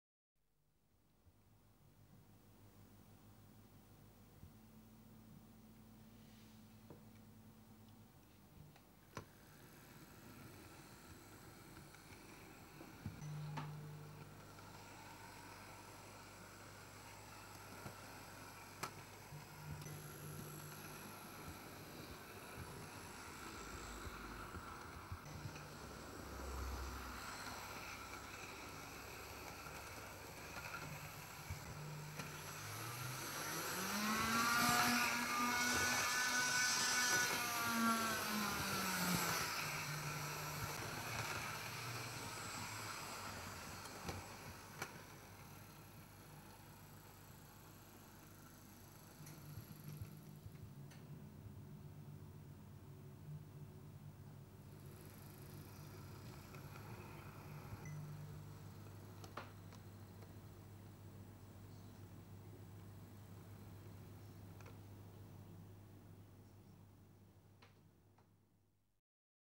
A1 Tornado on Test Track
So it can be seen and heard. Slow moving and over points too.

Amazing how quiet the newer Hornby five pole motors are.